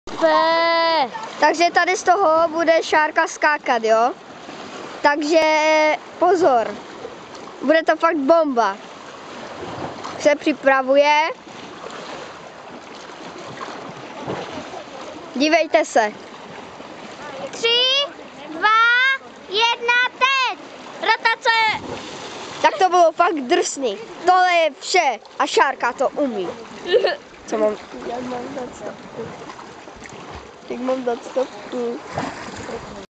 Černá Hora - skoky do vody - 3
Skokanské výkony naších dětí, Černá Hora